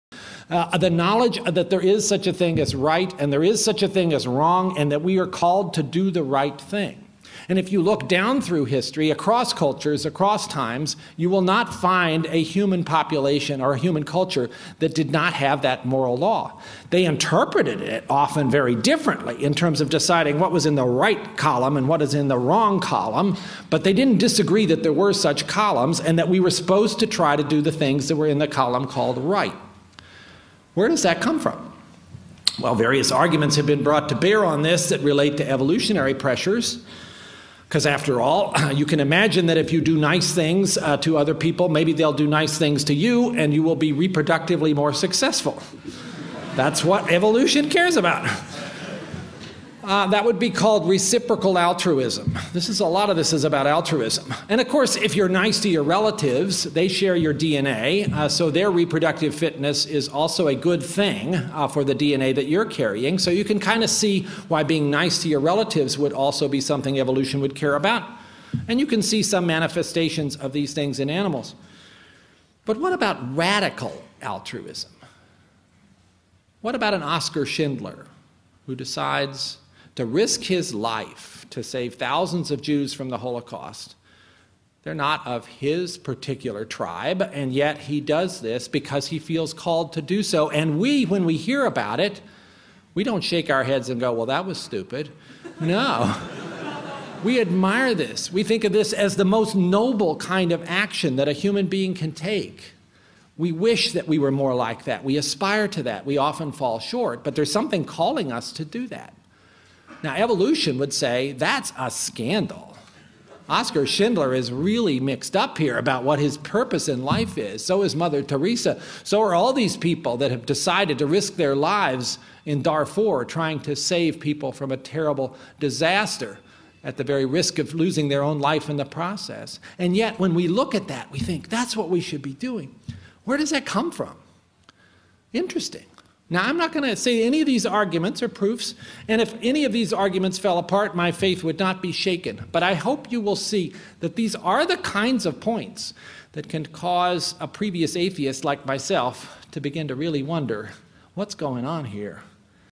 Evolution and Human Behavior - Where did our morality come from?
Dr. Francis Collins, director of the Human Genome Project.